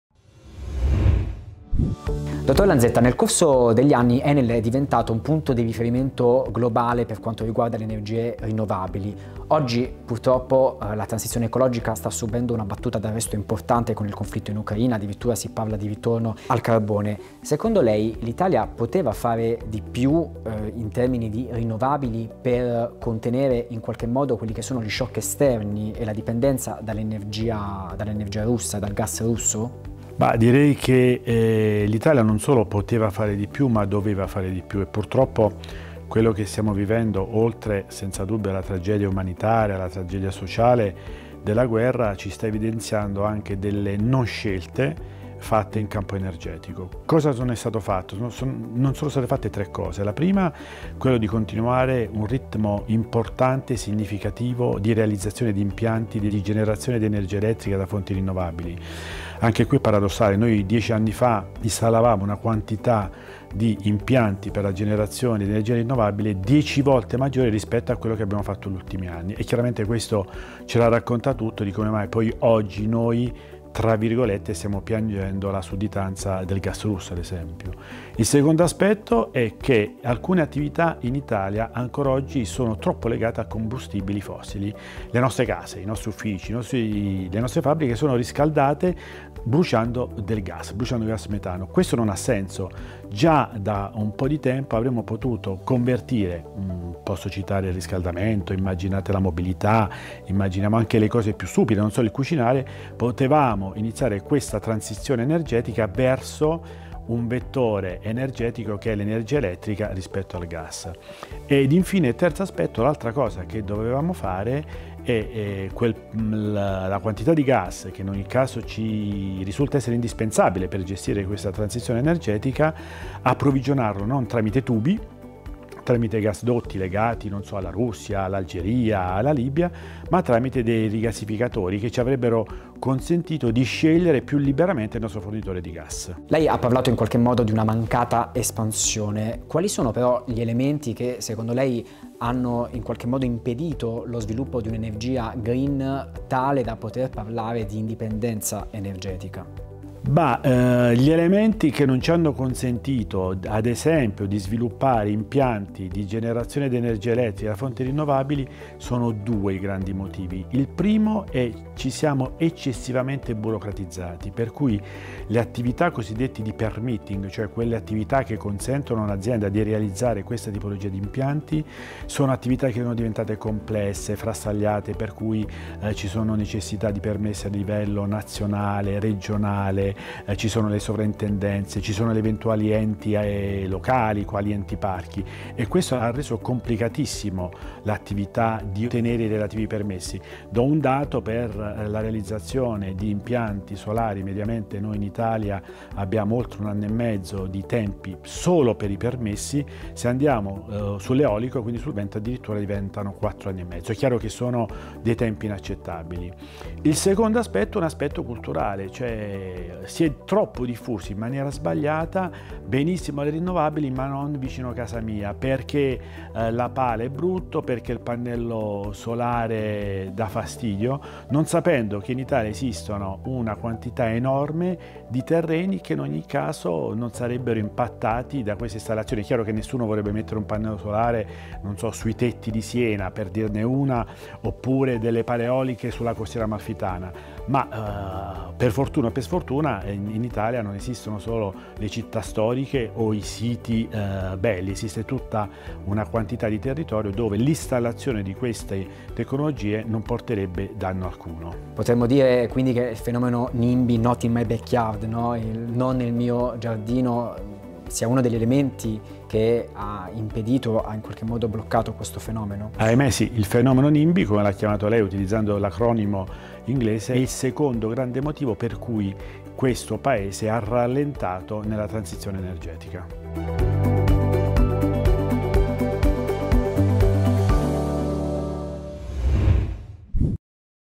Nicola Lanzetta (Enel): “Sull’energia l’Italia doveva fare di più per l’indipendenza dalla Russia”
Intervista di Antonio Santamato, giornalista Fortune Italia.
La guerra in Ucraina sta evidenziando plasticamente le “non scelte” dell'Italia fatte in questi anni in campo energetico. A confermarlo a Fortune Italia è Nicola Lanzetta, Head of Italy di Enel, che durante il convegno organizzato in collaborazione con il Centro Studi Americani il 21 aprile a Roma (“Gli Scenari Energetici: tra PNRR e Conflitto”) ha detto che, “l’Italia non solo poteva fare di più, ma doveva fare di più”. In particolare per il direttore Italia del Gruppo Enel, “non sono state fatte tre cose: in primis, continuare ad avere un ritmo significativo per la realizzazione degli impianti di generazione di energia elettrica da fonti rinnovabili. Paradossalmente dieci anni fa, per fare un esempio, l’Italia installava una quantità di impianti dieci volte maggiore rispetto a quella degli ultimi anni, mentre oggi piangiamo la sudditanza dal gas russo. Alcune attività del nostro Paese, inoltre, sono ancora oggi eccessivamente collegate ai combustibili fossili. Già da un po’ di tempo avremmo potuto convertire i nostri consumi, iniziando la transizione energetica verso l’elettrico, anziché il gas. Infine – dice Lanzetta -  avremmo dovuto approvvigionare il gas tramite rigassificatori, e non attraverso i tubi. Questo ci avrebbe consentito di scegliere più liberamente il nostro fornitore di gas”. Una cosa che oggi, purtroppo, non è possibile e che stiamo pagando caro.

Ma quali sono gli elementi che ci hanno impedito di andare dritti verso una transizione energetica più veloce? Per Lanzetta nel tempo “ci siamo eccessivamente burocratizzati. Attività complesse e frastagliate, necessità di permessi nazionali, regionali, addirittura degli enti locali, hanno reso complicatissima l’attività per ottenere i relativi permessi. Mediamente solo per le autorizzazioni degli impianti eolici servono quattro anni e mezzo. Tempi inaccettabili! Il secondo aspetto, poi, è culturale: si è diffuso in maniera sbagliata il fenomeno NIMBY (not in my backyard, non nel mio gradino, ndr), non sapendo che in Italia esiste una quantità enorme di terre che, in ogni caso, non sarebbe impattata da queste installazioni”.